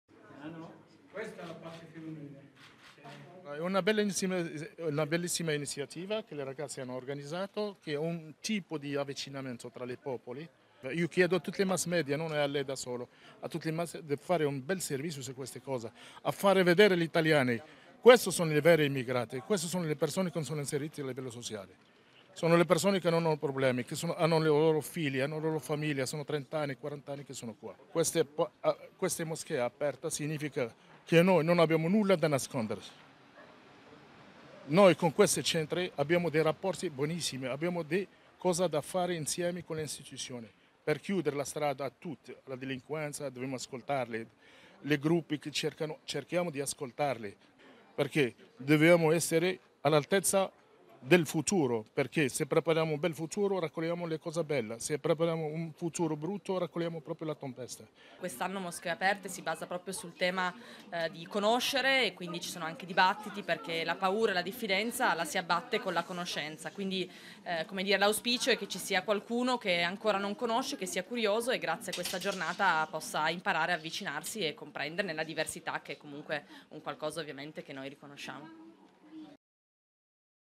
A Torino una giornata con le moschee aperte: "Paura e diffidenza si abbattono con la conoscenza"
Per il terzo anno consecutivo torna l'iniziativa "Moschee aperte", una giornata in cui i centri islamici della città accolgono tutti i cittadini di qualunque religione. Ad aprire le visite la sindaca, Chiara Appendino che si è recata nel pomeriggio nella moschea di piazza Cattaneo nel quartiere di Mirafiori. "Quest'anno l'iniziativa si basa sul tema della conoscenza perché la paura e la diffidenza si abbattono con la conoscenza "  " Ha sottolineato la prima cittadina "  " La speranza è che qualcuno sia curioso e che con questa giornata possa avvicinarsi e comprendere la diversità che è un qualcosa che noi riconosciamo". .  .Di Alessandro Contaldo